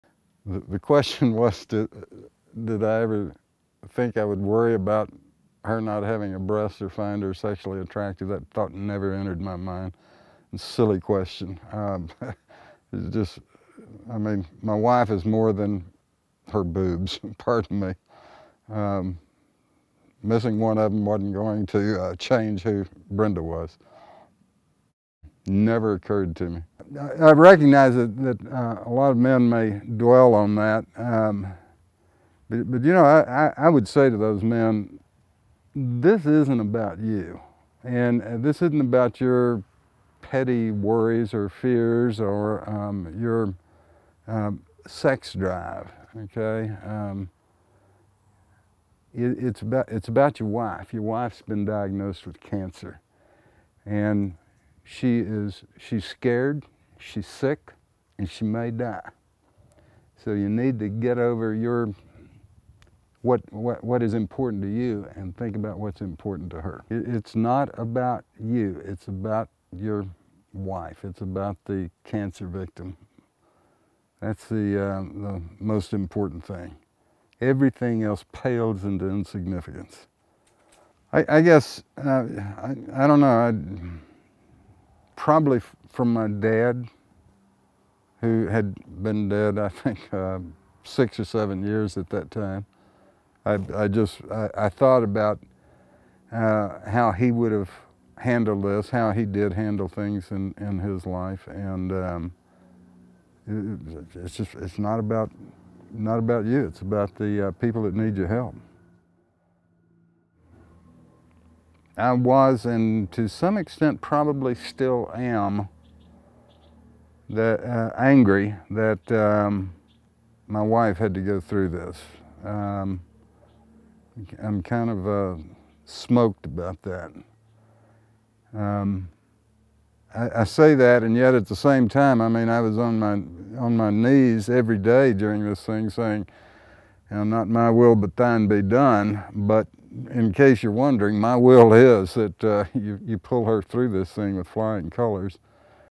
Breast Cancer Advice for Husbands
James Coffee gives advice to other breast cancer husbands. This is not about you, but about your wife.